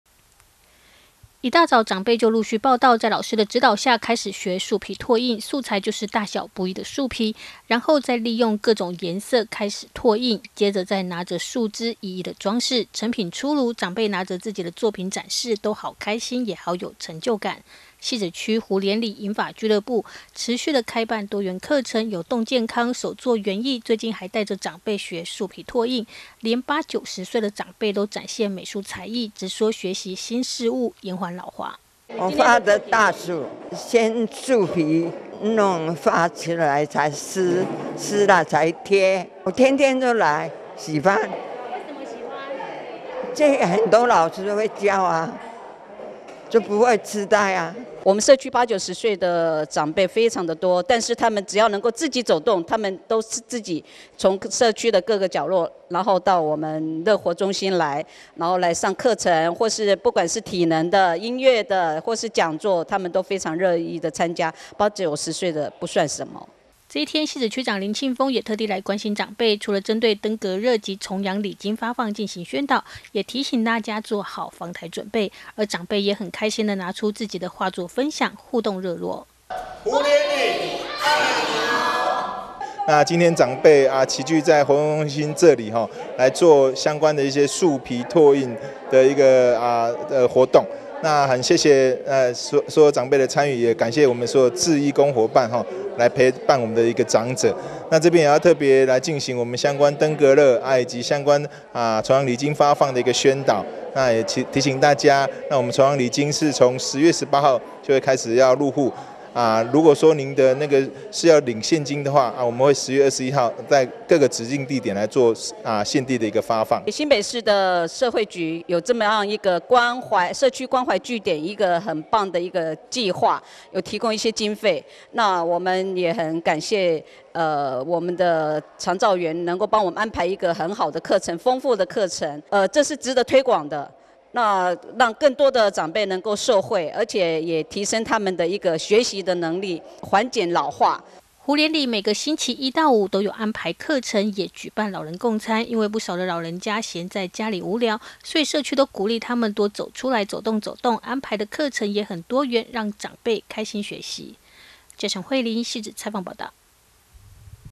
20231005觀天下新聞 學新事物延緩老化 湖蓮長輩學樹皮拓印
鼓勵長輩多走出來運動也學習新事物，汐止區湖蓮里銀髮俱樂部持續開辦多元課程，有動健康、手作園藝，最近還帶著長輩學樹皮拓印，汐止區長林慶豐也特地前往關心長輩，做政令宣導也一起吃飯話家常。